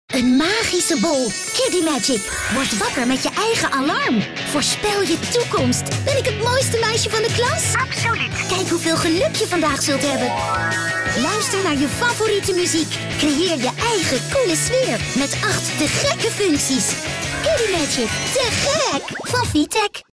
Vtech - Kidimagic
De Vtech KidiMagic is meer dan een eenvoudige wekker... Het is een magische bol met eindeloos veel mogelijkheden!